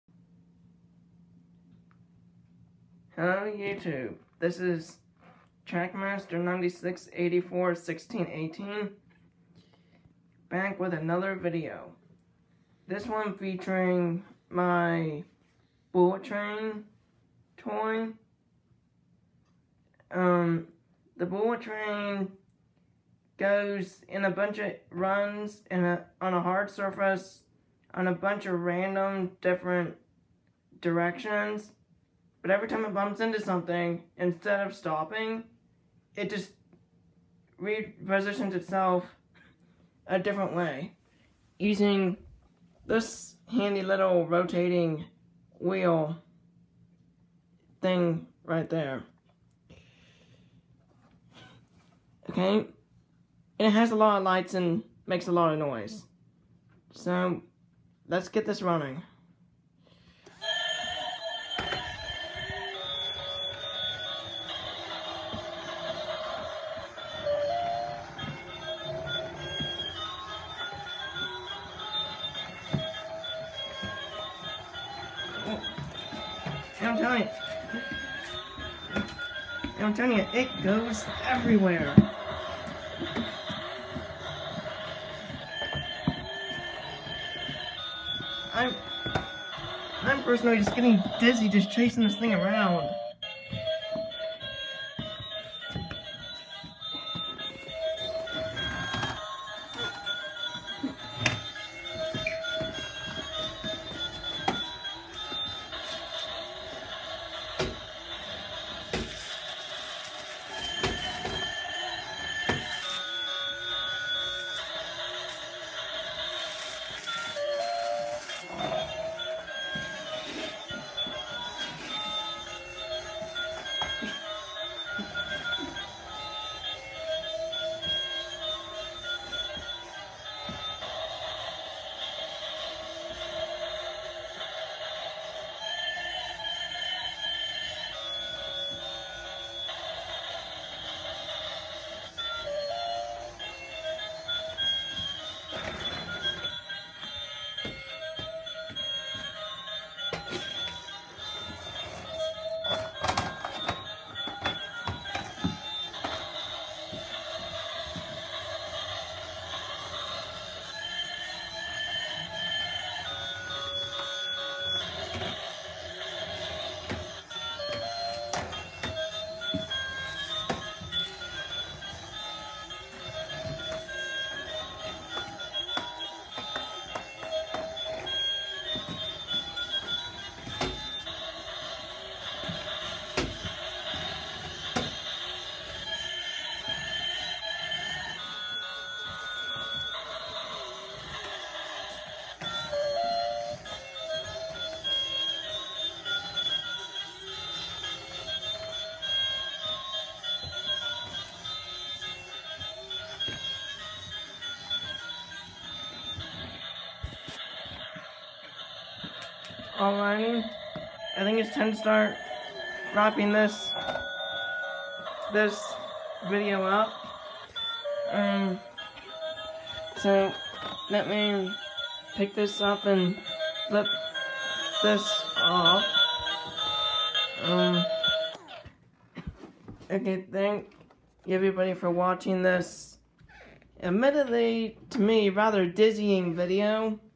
Bullet Train run video
filmed with my iPad Air 2. Camera people were me at first then i passed off to my attendant when i started having balance issues chasing that train around the kitchen. The other pair of feet (with the boots on) do belong to my attendant, the ones in tennis shoes are mine. This video was fun and very exhausting to make (it took me 3 tries!) I enjoyed it though and I hope you will too.